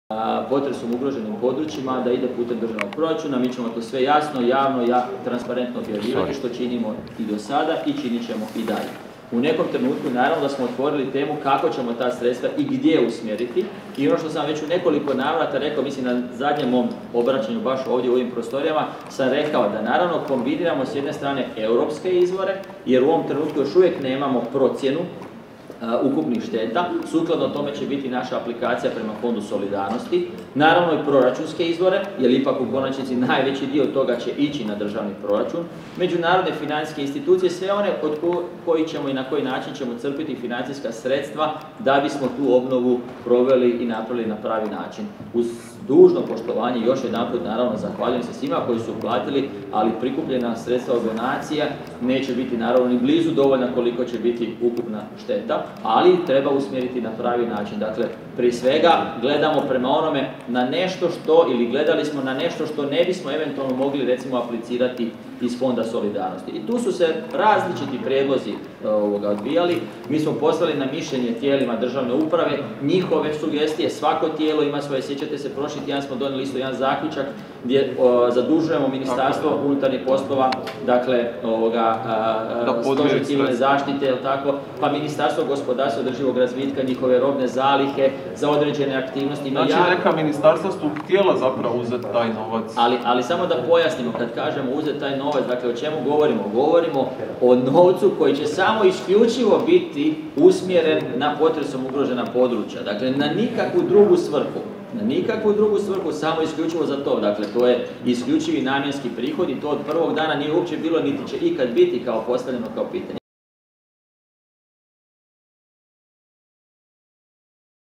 Ministar Marić o raspodjeli doniranih novaca skupljenih za pomoć potresom pogođenim područjima
Nakon završetka Vlade ministar financija Zdravko Marić održao je konferenciju za medije na kojoj je govorio o raspodjeli doniranih novaca skupljenih za pomoć potresom pogođenim područjima